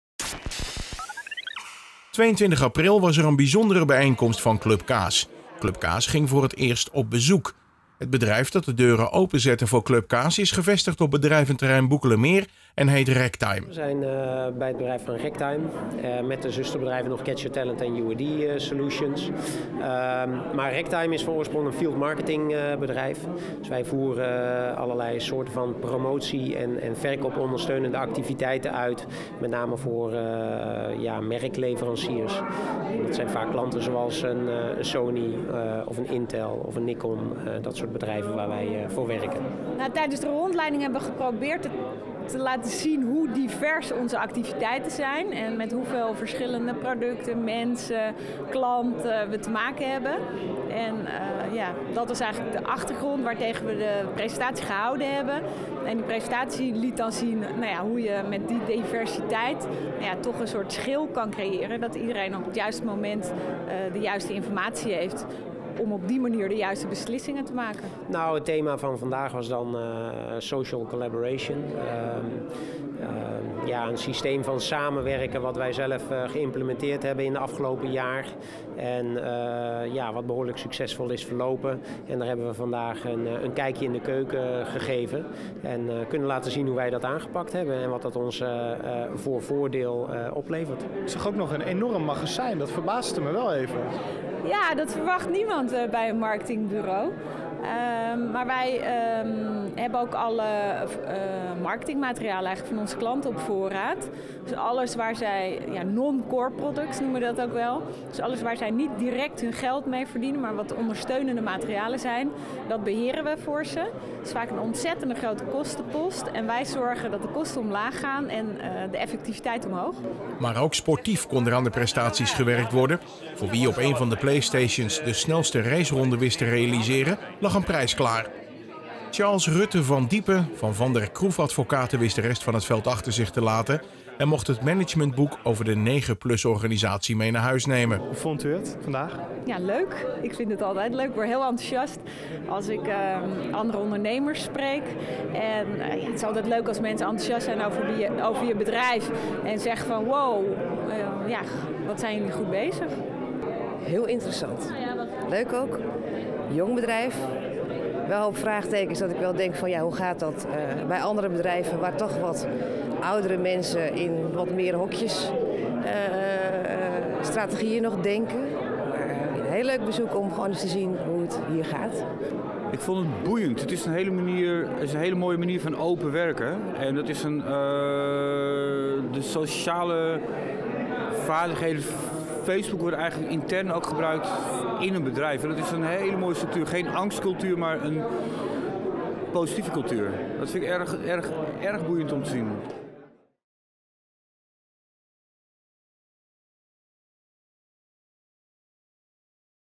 CLUB CAAS Bedrijfsbezoek Racktime
Racktime in Alkmaar opende haar deuren voor clubcaas, Hans Knipscheer en onderneemster van het jaar Marie-Jose van den Kommer vertellen zeer openhartig hoe zij ondernemen met behulp van onder andere social collaboration en de 9+ organisatie.